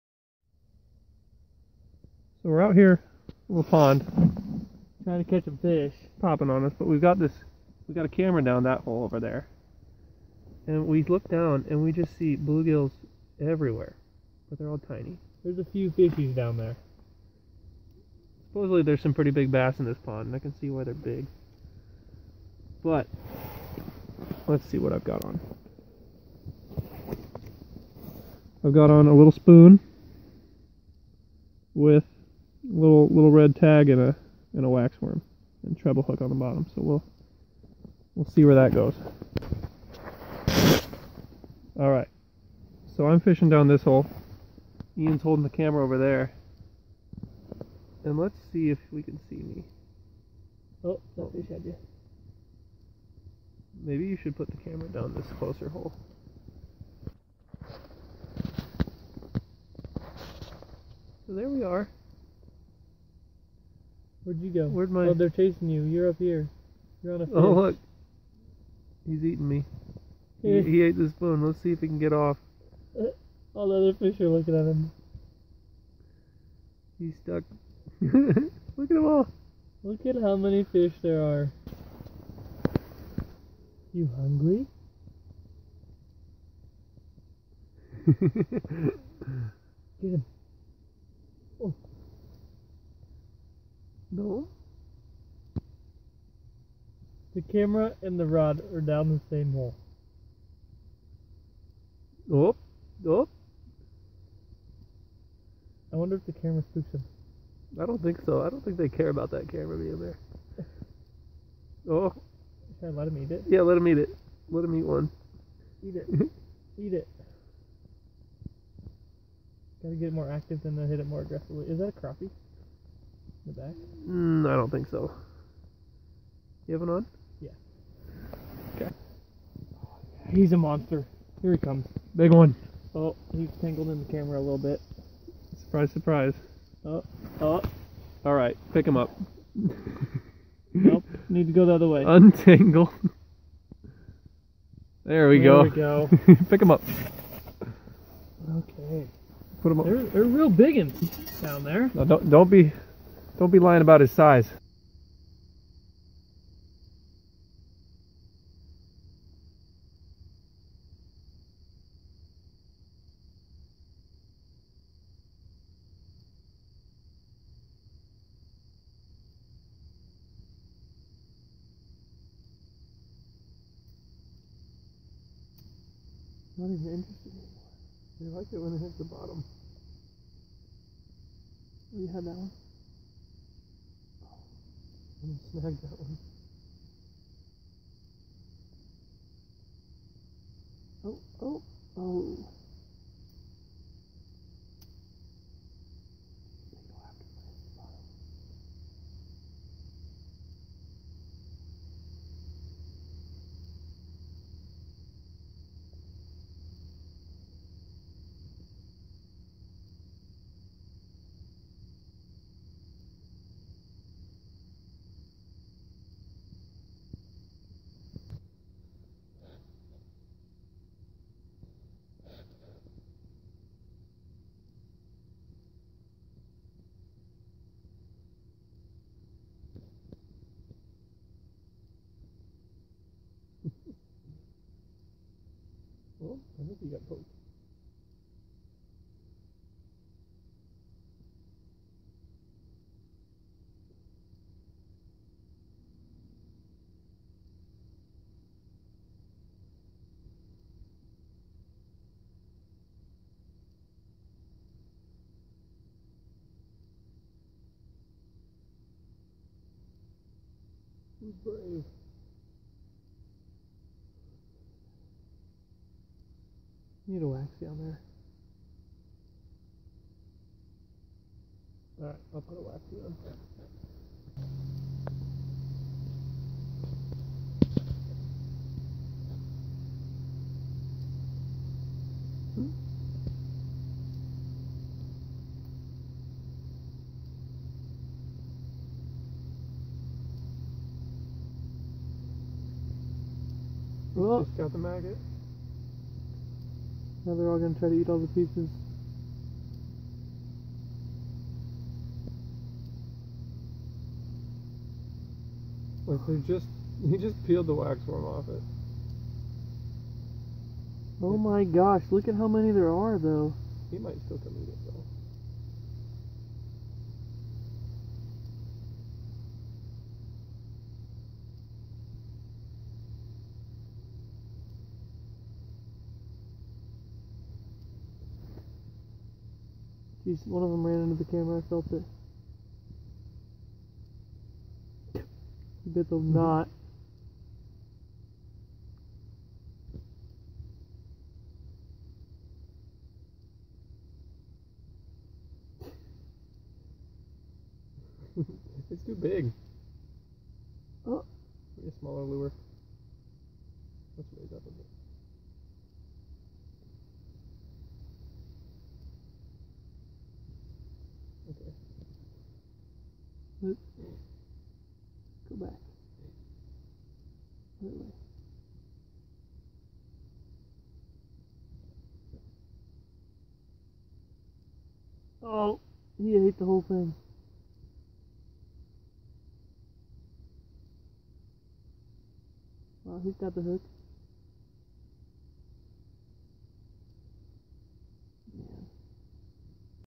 Ice Fishing | FEEDING FRENZY 1/5/18
Ice fishing a local pond. We were targeting the big largemouth bass but only found these little green sunfish and bluegills. They were sure hungry!  1/5/18